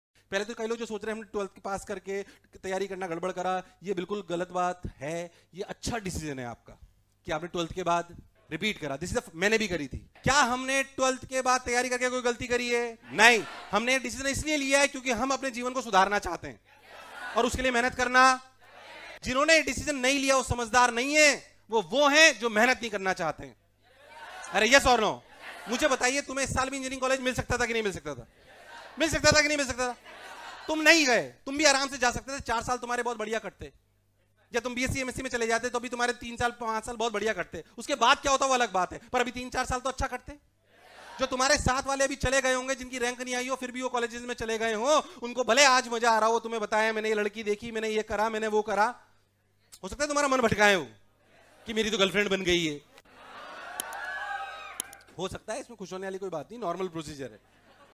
Drop lena galat decision hai! | NV SIR Shorts | Motion JEE
A drop year is a crucial year for the JEE\NEET Aspirants, and many a times these aspirants feels demotivated by the thought of it. In this video NV Sir tells his students that how taking a year can be beneficial too and that there is nothing wrong in it.

Register here Hindi Medium batch.